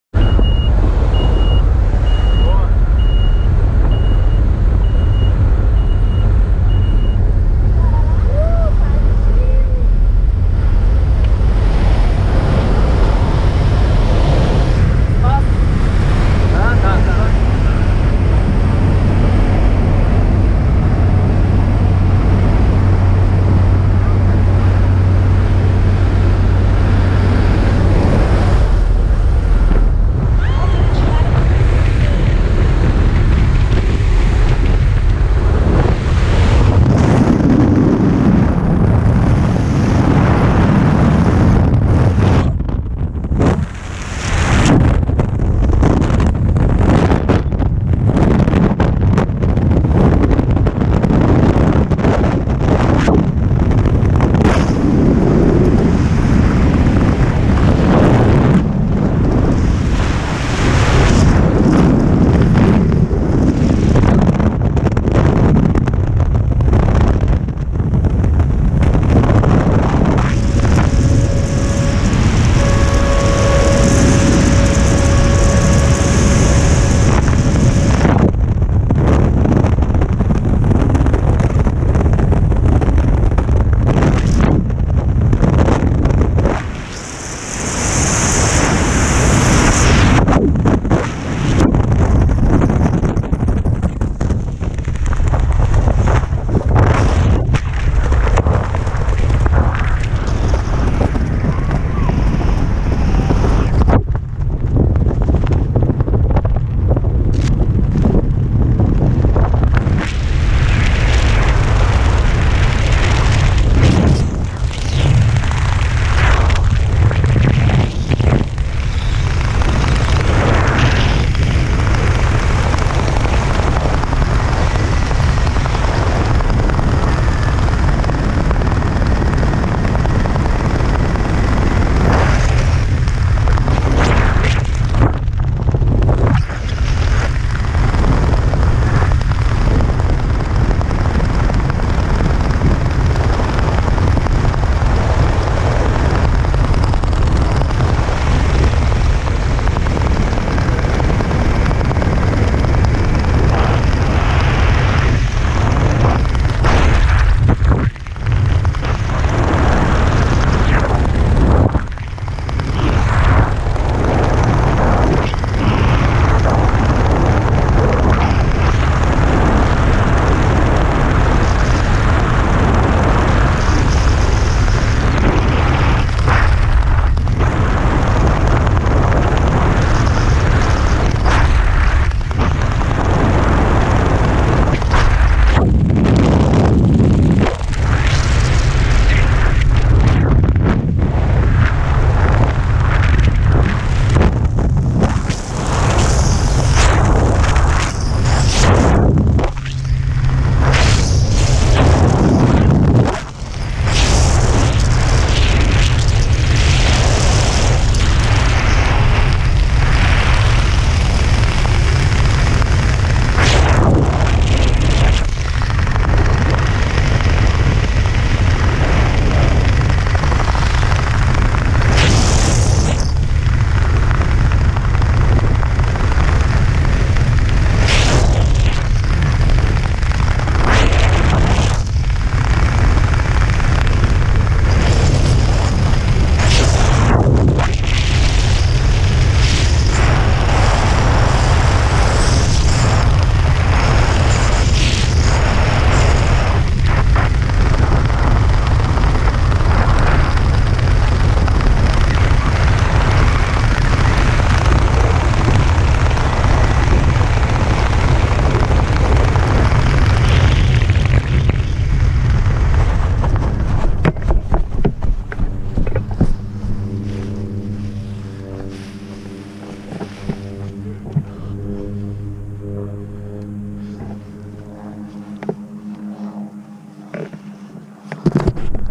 Level training BBF skydive - my camera
BBF training at CéuAzulParaquedismo Boituva... Training the adjustment of level, but not very successfully...